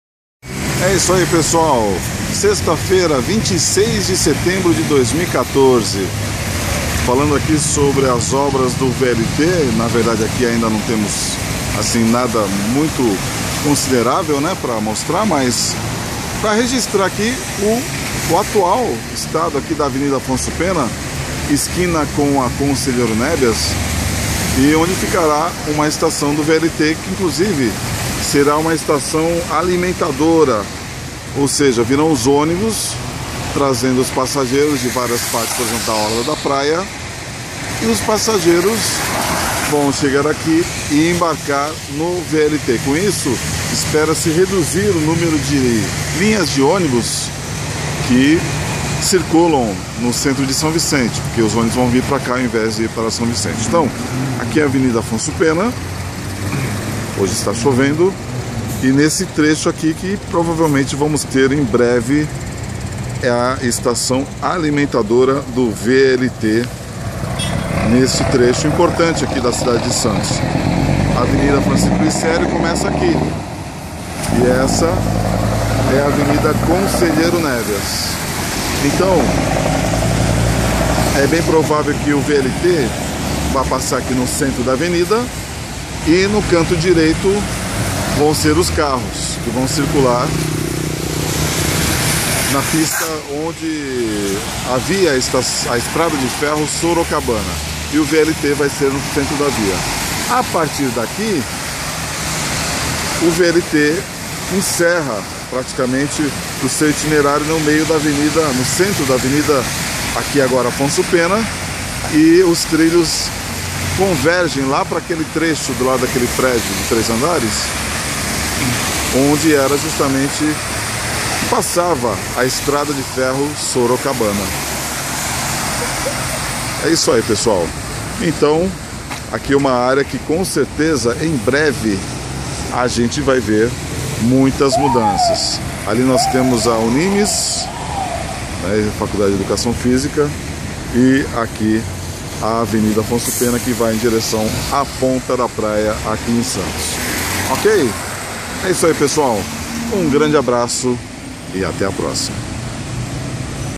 26.9.2014 VLT e a futura estação na Afonso Pena
Aqui neste local ficará uma estação do VLT e onde também será o embarque de passageiros vindos das linhas de ônibus alimentadoras.